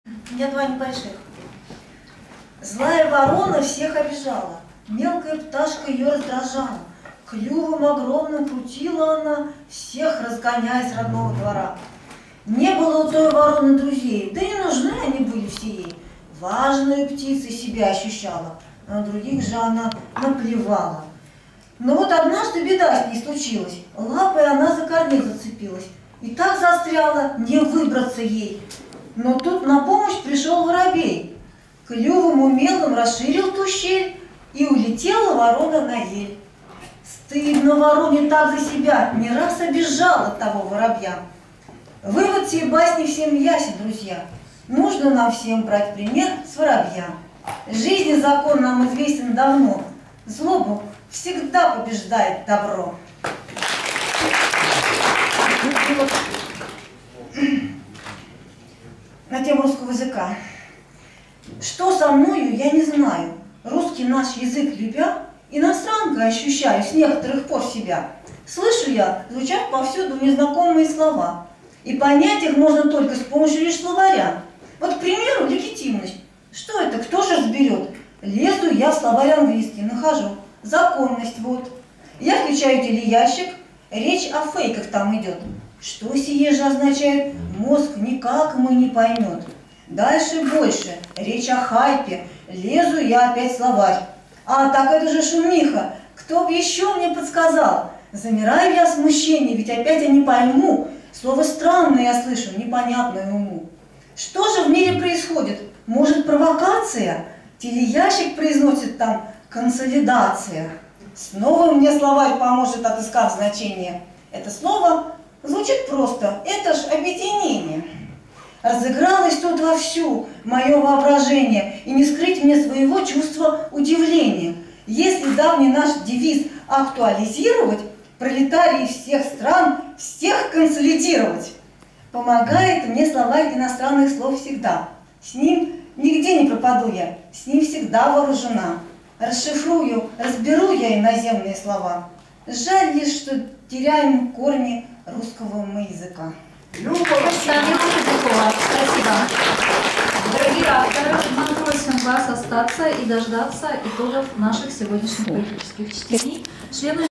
Я читаю свои стихи в ЦДЛ: басня "Злая ворона" и иронические стихи "Странности современного языка"
Я читаю свои стихи в Центральном Доме Литераторов на поэтических чтениях: басня "Злая ворона" и иронические стихи "Странности современного языка".